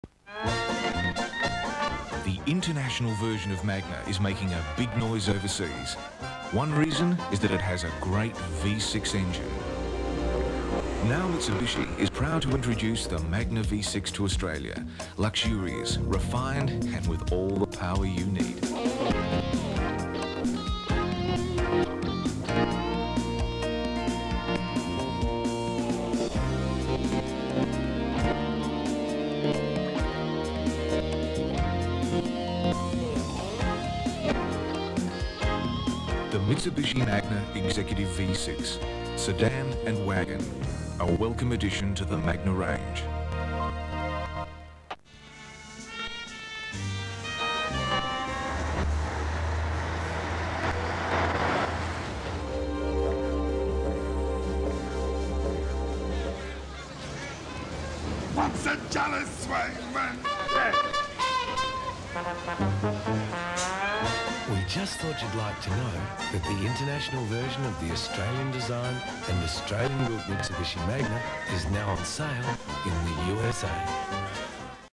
Mitsubishi Magna commercials (1993)
Two commercials combined from 1993 for the Mitsubishi Magna.